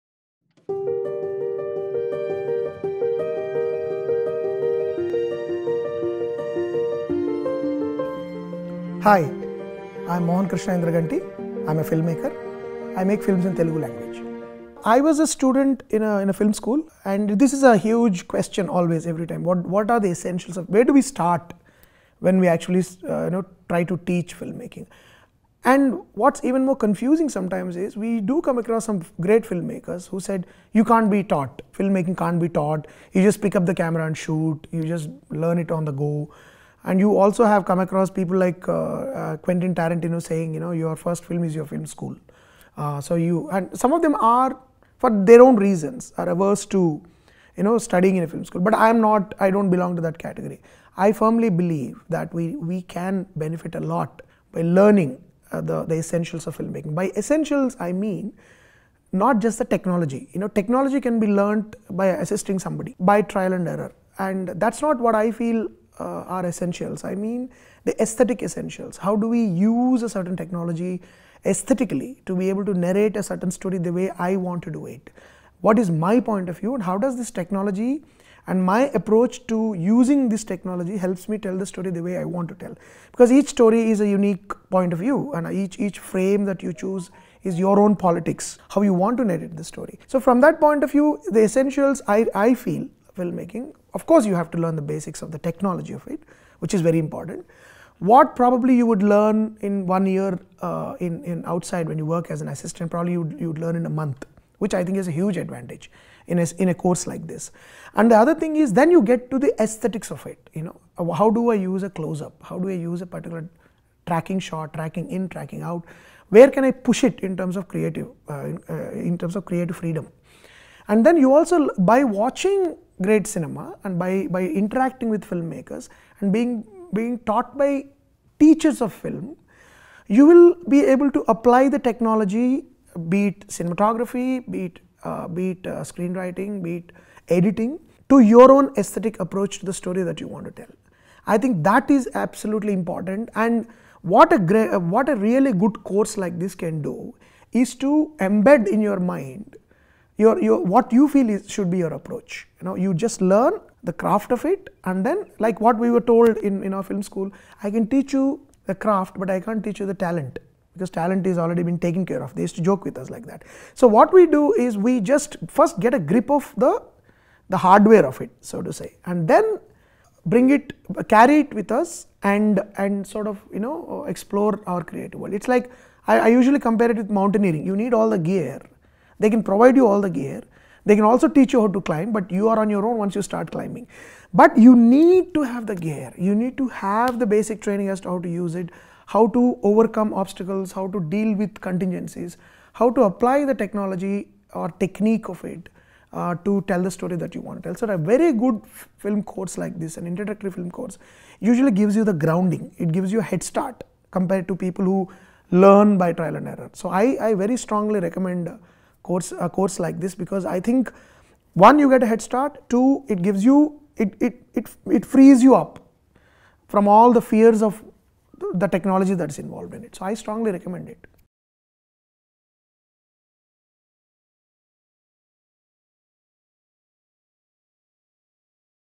Essentials of Filmmaking - Online Course
The Online Certificate Program- Essentials of Filmmaking is a Program aimed at students who aspire to embark upon a career in Filmmaking.

This course has been developed by Annapurna College. The course covers six key modules- Production, Script Writing, Cinematography, Editing, Sound Design and Ethics in Filmmaking. Each module has been curated by Film Academia and is based on insights shared by Film Industry Experts.

This program provides aspiring filmmakers an introductory window to the world of Media and Entertainment. With this program, students learn the basics of Filmmaking process which sets the foundation for deeper Film and Media Education. Students can further hone these primary learnings through practical training and by working as part of a Film Crew.

This is a Beginner Level, 50-Hour self-paced online program which gives students the flexibility to take the course anytime and from anywhere.

During the course, Students have the access to a well-researched repository of online learning videos anchored by some of the best minds in Film Industry, receive constructive feedback on their assignments from experienced Faculty (via chat / email) and gain the opportunity to attend expert webinars organized by the College on a periodic basis. Students can refer the courseware via an Online Library accessible for a period of 4 months from the time of registration and will have the convenience of completing the 50-hour course anytime and from anywhere during this period.

Or call our counsellor at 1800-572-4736